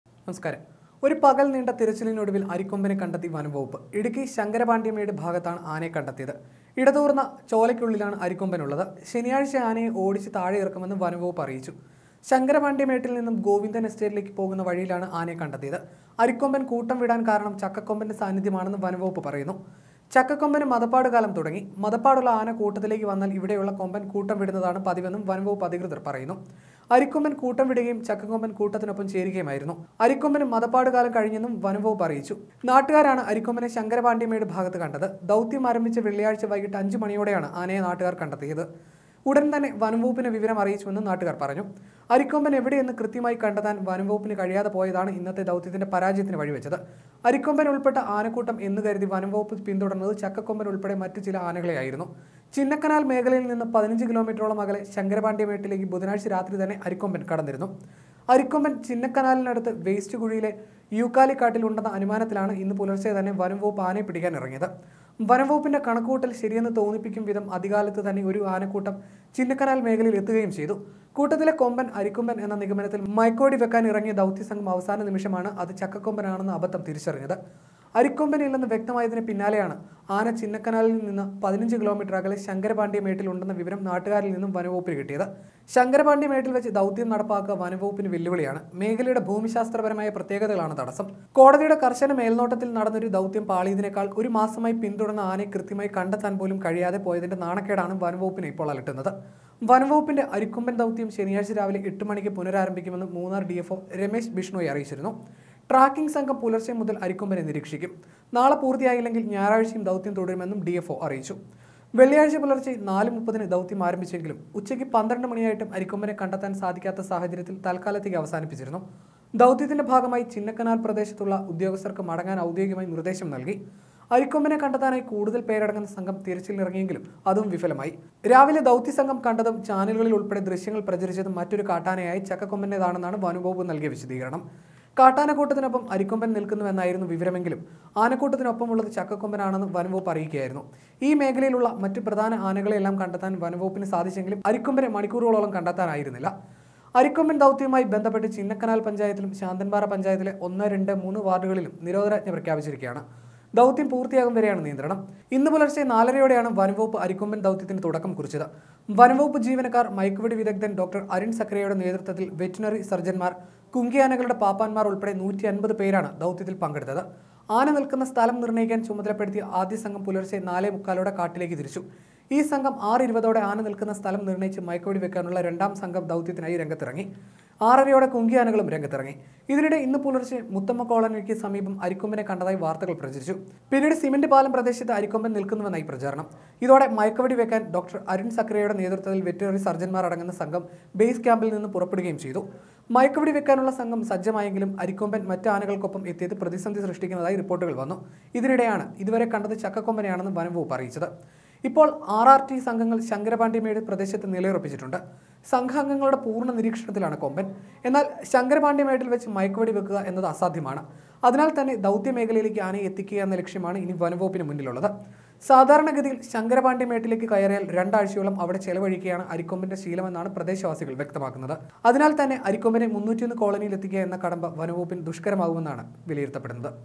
അരിക്കൊമ്പനെ ശങ്കരപാണ്ഡ്യമേട്ടില്‍ കണ്ടെത്തി.. ദൗത്യം നാളെ തുടരും I Arikomban
ചക്കക്കൊമ്പന് മദപ്പാട് കാലം; ആനക്കൂട്ടത്തെ വിട്ടകന്ന അരിക്കൊമ്പനെ കണ്ടെത്തിയത് ശങ്കരപാണ്ഡ്യമേട് ഭാഗത്ത്; സ്ഥലം തിരിച്ചറിഞ്ഞത് ഇന്നത്തെ ദൗത്യം അവസാനിപ്പിച്ച ശേഷം; മേഖലയുടെ ഭൂമിശാസ്ത്രപരമായ പ്രത്യേകത ദൗത്യത്തിന് തടസ്സം; ആനയെ ഓടിച്ച് താഴെയിറക്കുമെന്ന് വനം വകുപ്പ്